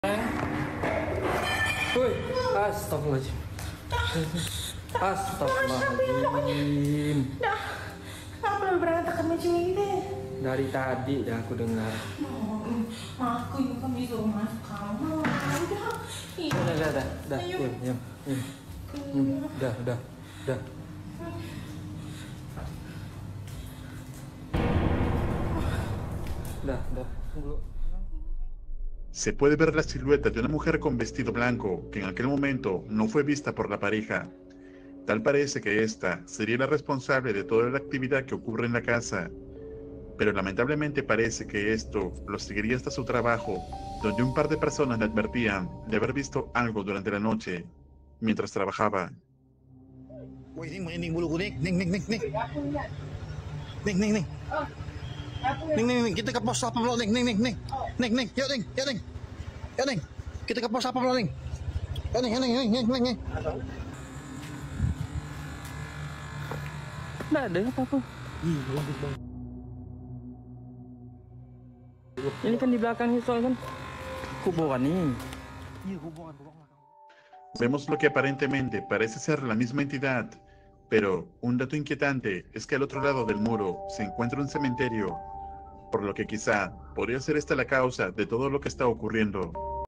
Siluetas Misteriosas ¿Qué Oculta la Casa 👻
Una mujer vestida de blanco acecha la casa